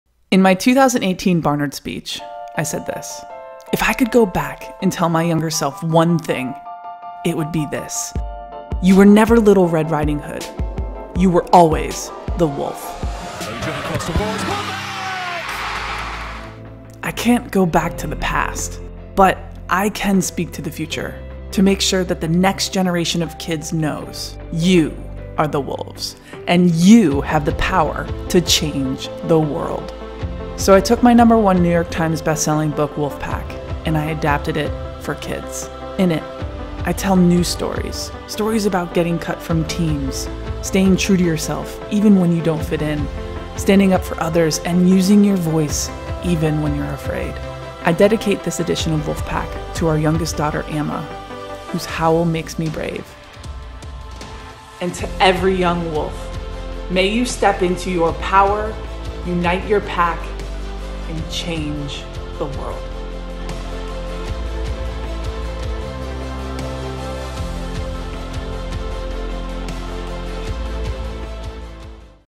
Wolfpack Young Readers Edition by Abby Wambach | Preorder Trailer
In this young readers adaptation of her #1 New York Times bestselling book, two-time Olympic gold medalist and FIFA World Cup champion Abby Wambach inspires the next generation to find their voice, unite their pack, and change the world.